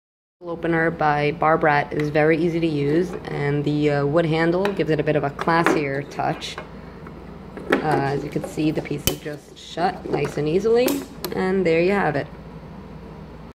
Bar brat bottle opener all in one
I really love this bottle opener/cork screw! The wood handle gives it a bit of a more upscale, classy look, making it the perfect tool to bring out during dinner parties, when fancy wines, etc are being served. It works great, also, which is of course the most important part. I have tried the foil cutter, corkscrew, and bottle opener - all of which worked exactly as expected. I have tried many products by Bar Brat and they have never disappointed me. This product, like their other products, is well made and durable, which makes me think it will last me a long time!

I received this product in exchange for an honest and unbiased review.